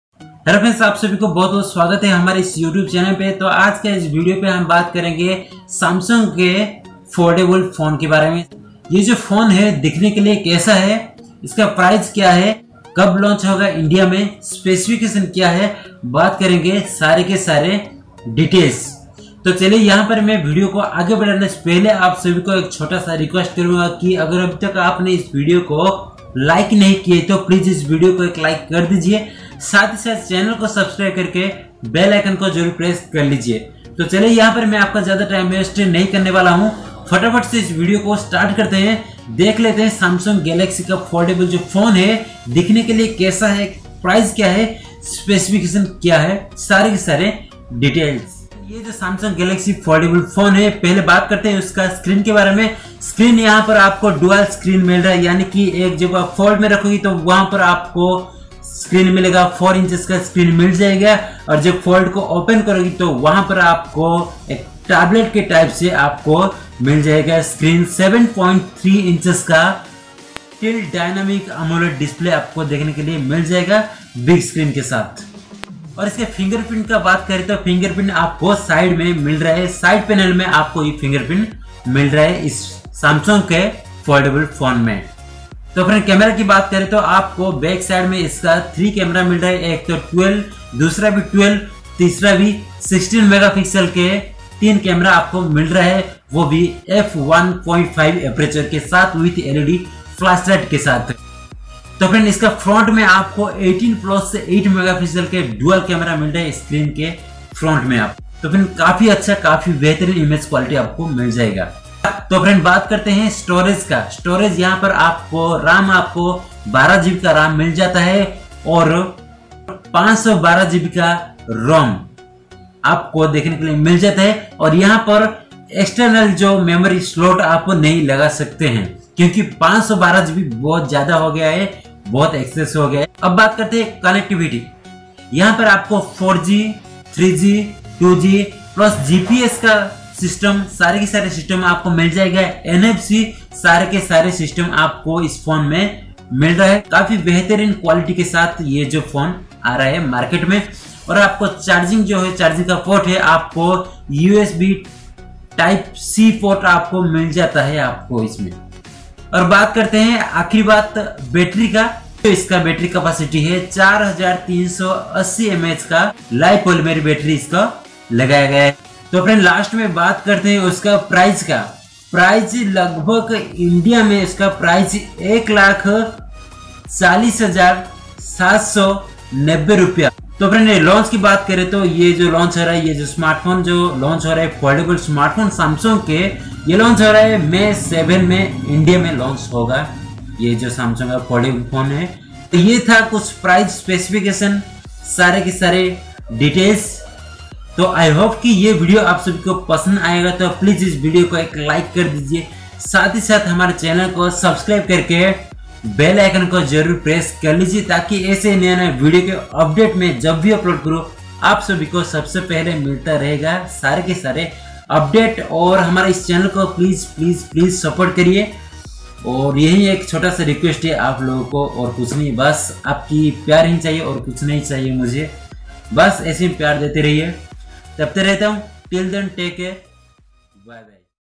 Samsung Galaxy fold Lunch Date Price full Details
Dosto is vdo pe Maine Samsung galaxy fold ke full details ke bareme bataya hu